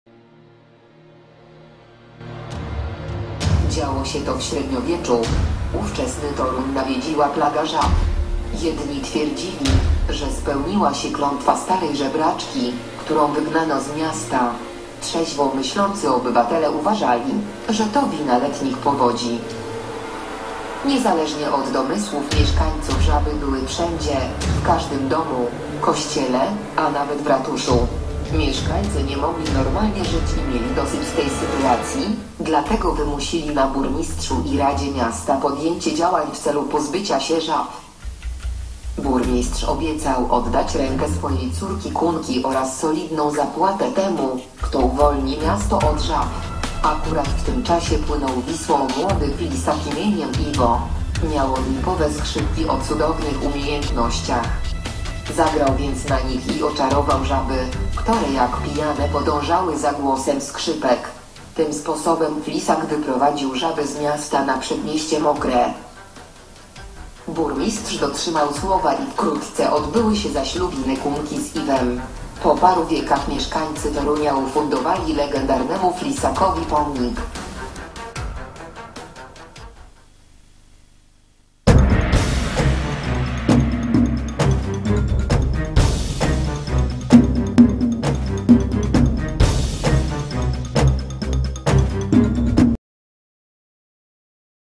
O flisaku i żabach
Auorka: Małgorzata Roznowska. VI LO Toruń.
Film opowiada legendę toruńską o flisaku i żabach, która głosi, że pewnego roku na skutek powodzi lub klątwy Toruń nawiedziła istna plaga żab. Mieszkańców uratował flisak o imieniu Iwo. Jego pomnik stoi na toruńskim Rynku.
Film możliwy do wykorzystania na zajęciach z historii, języka polskiego, wok.
Konkurs filmowy w projekcie (Ro)Zoom na Kulturę - realizowany w ramach programu Fundacji Orange przez Fundację Think!